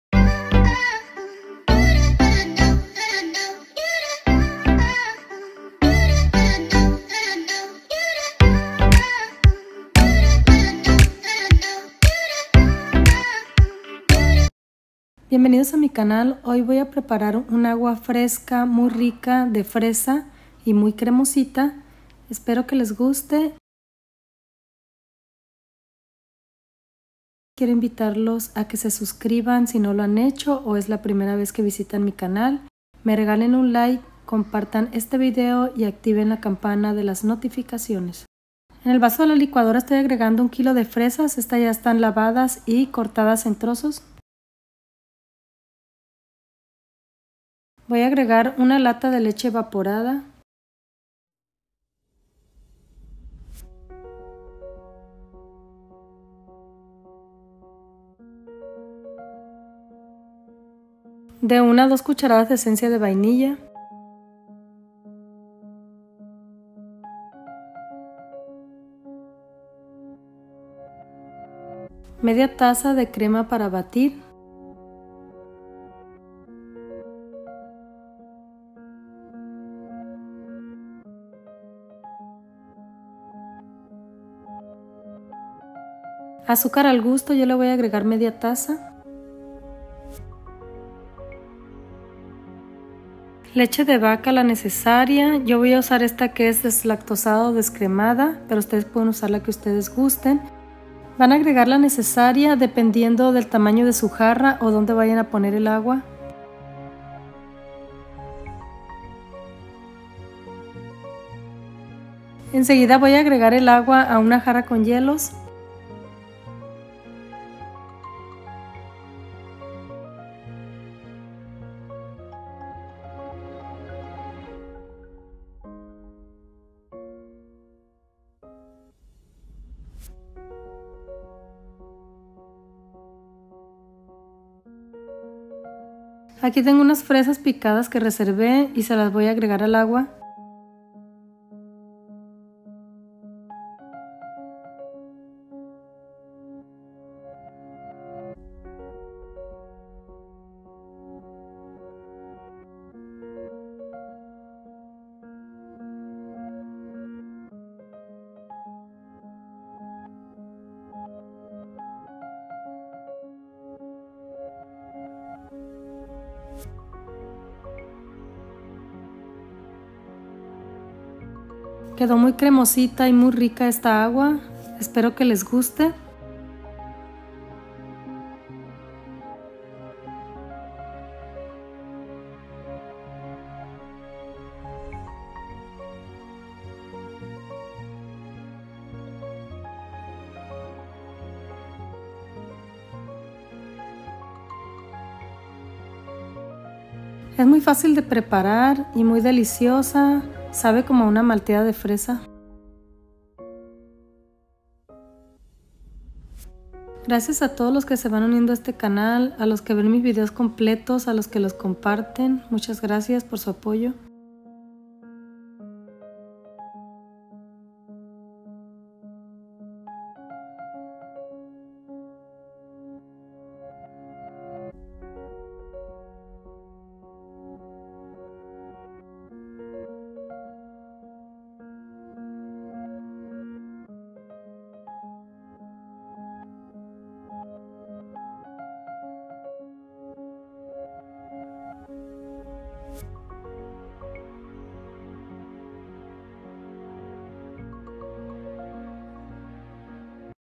AGUA DE FRESA 🍓 SÚPER CREMOSITA 😋
Una rica agua de fresa cremosa,muy fácil de preparar y a todos les encanta.